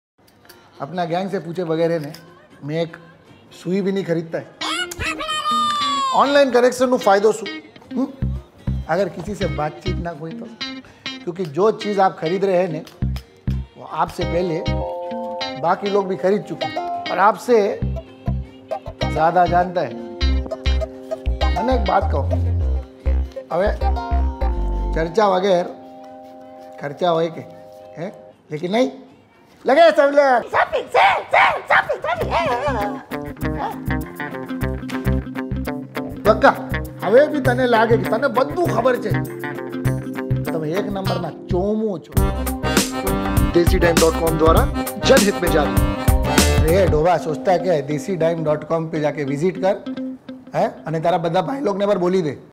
Save money!! When you Shop Online. Aap Chomu Hai : Ad by Desidime, Aapki Online Shopping Gang!
This  is an attempt to encourage people to do a little 'Charcha' before doing 'Kharcha' on Desidime.com, India's Biggest Online Shopping Community.

When you visit Desidime.com there are thousands of offers and discounts from the best ecommerce websites like Flipkart, Amazon, Snapdeal, Paytm, Craftsvilla & More.